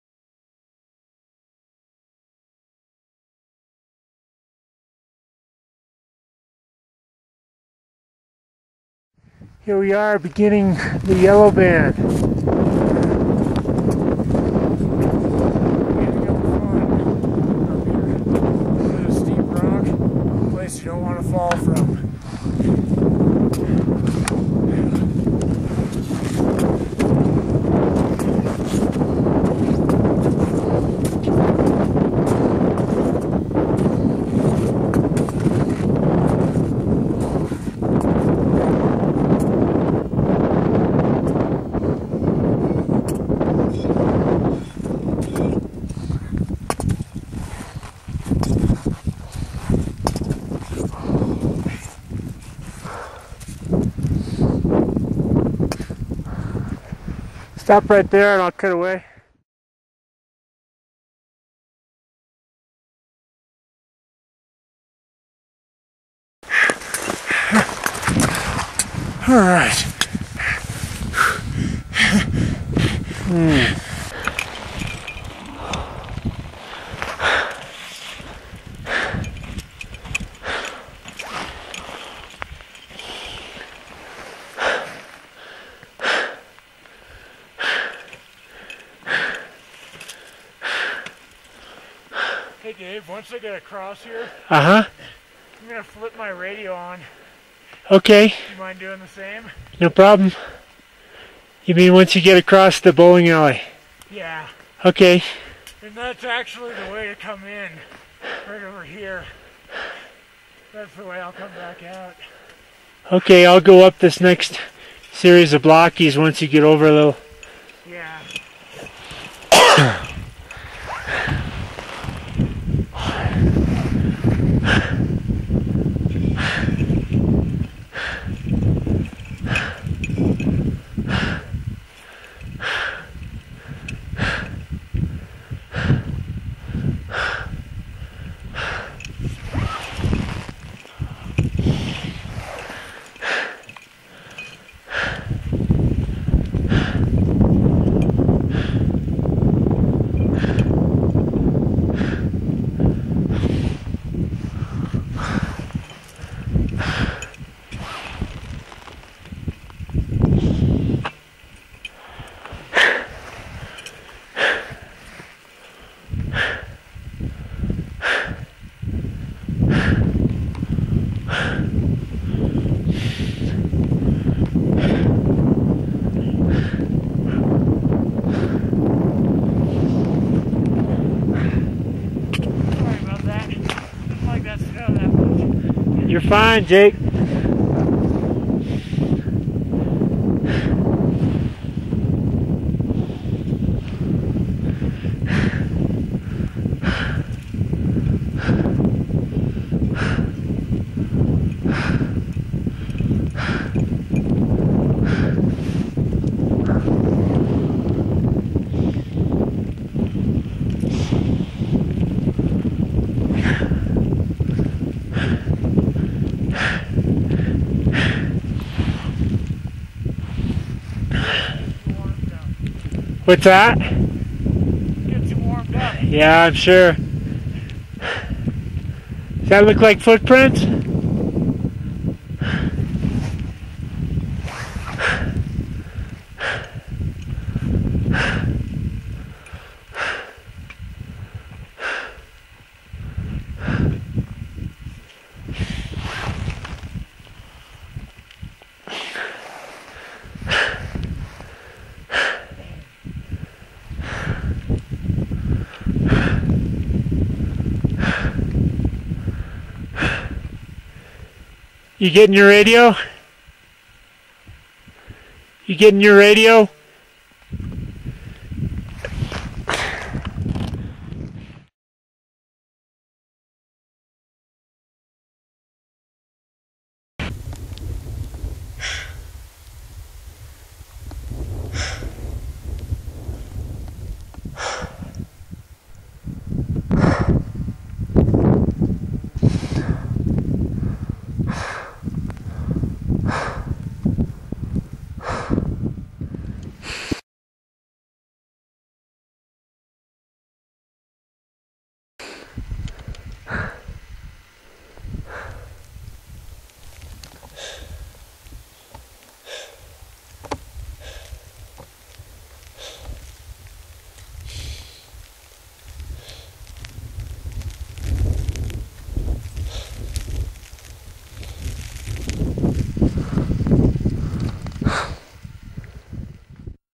The Search for Andrew Irvine - 2004
Jake Norton & Dave Hahn search the Yellow Band high on Mount Everest in the spring of 2004 looking for signs of Andrew Irvine who disappeared with George Mallory on June 8, 1924, high on Everest. for more information!